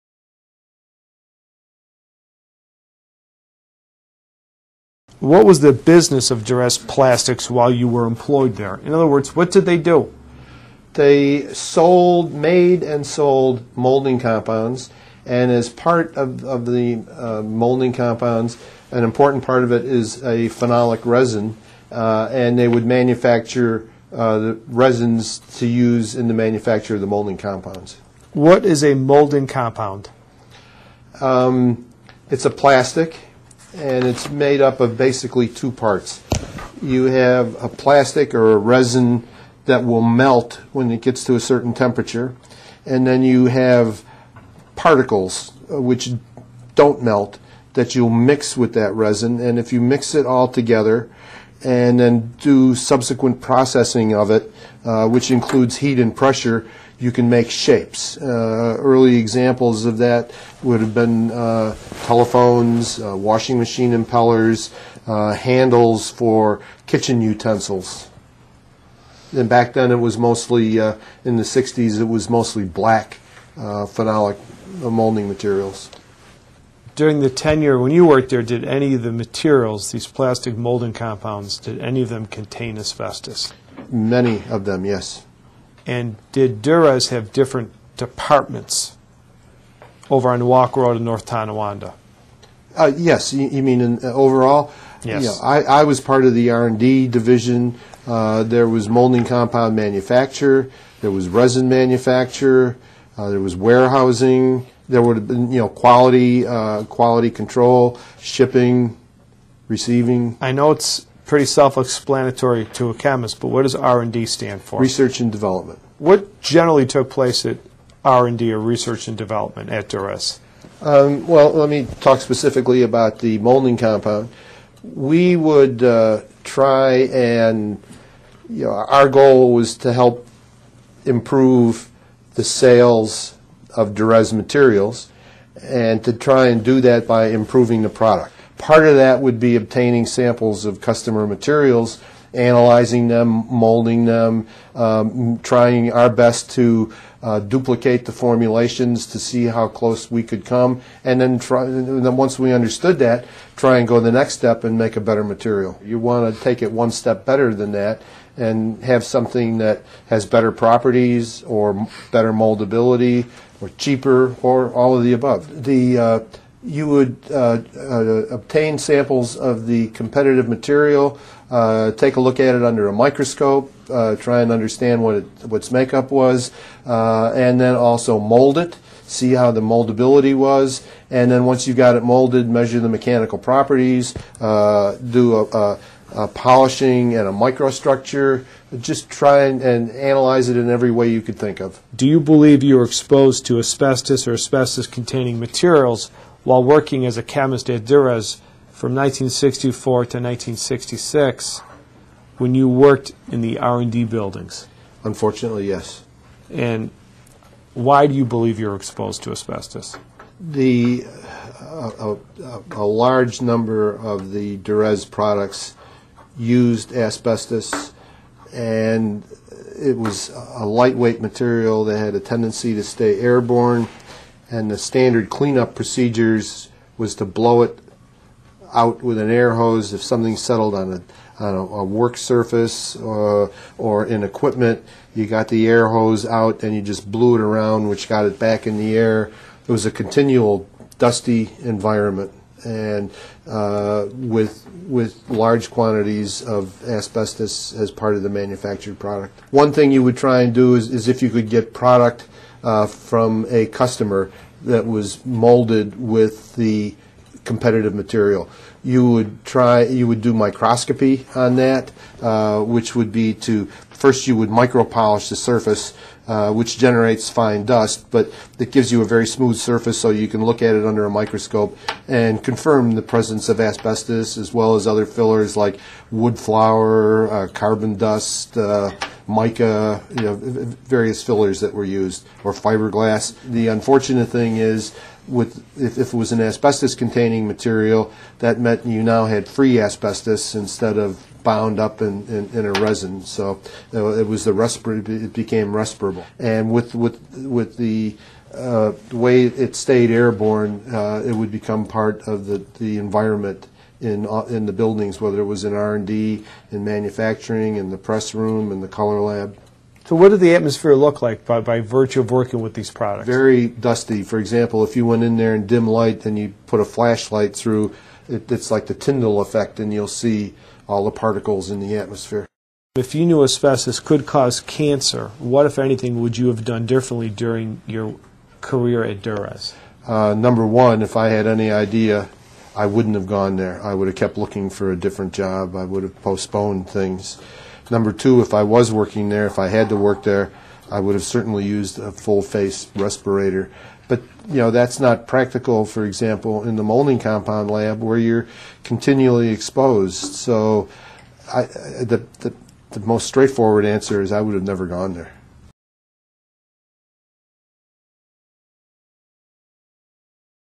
Client Describes His Exposure to Asbestos While Working as a Chemist at Durez Plastics
Our client worked for two years as a chemist in the Research and Development department at Durez Plastics in North Tonawanda, New York. In the above video, he describes his exposure to asbestos-containing plastic molding compound during his employment at Durez.